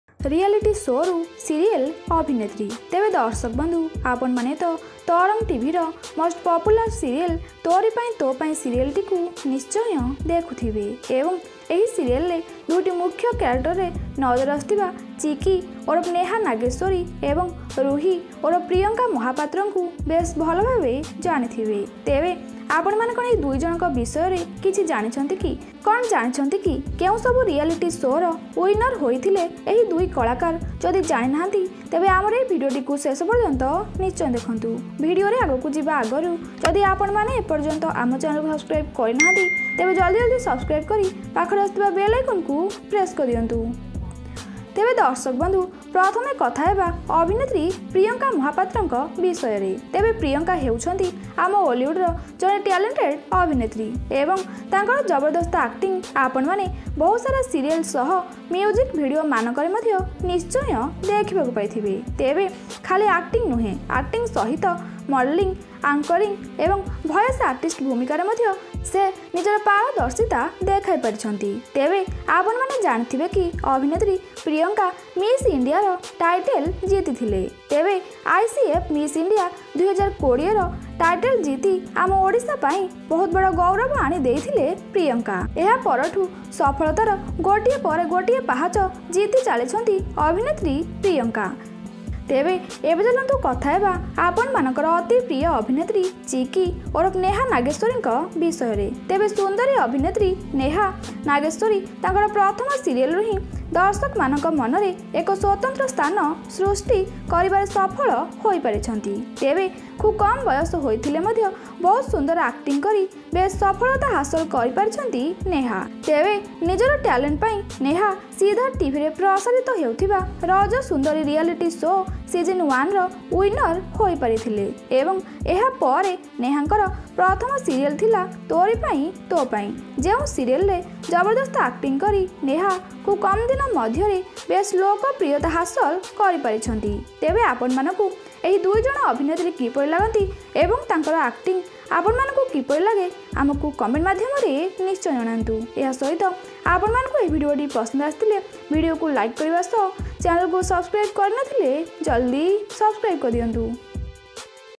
Tori Pain To Pain Serial Actress Neha Nageswari & Priyanka Mohapatra Real Life Success Story
OLLYWOOD IDEA

JAY JAGANNATH🙏